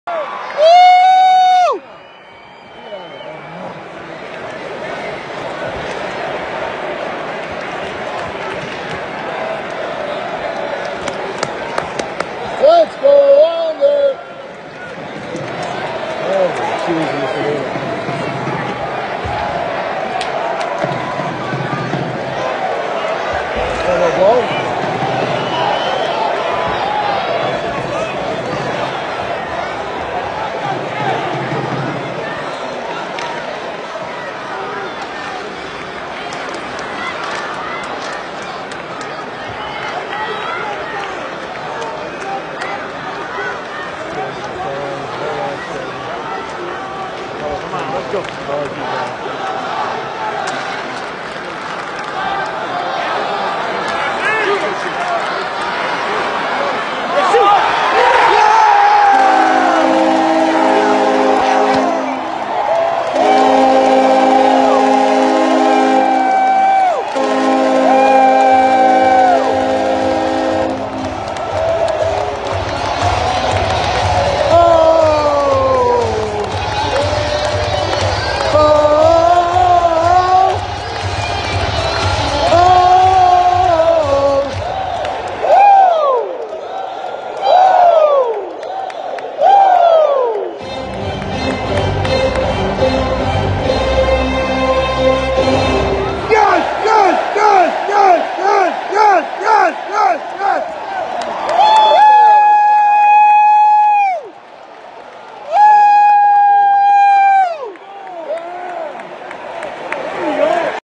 Mat Barzal Game Winning Goal January 13 2022 Devils at Islanders UBS Arena
3-2

15:07 3rd period

Assisted by Bailey & Mayfield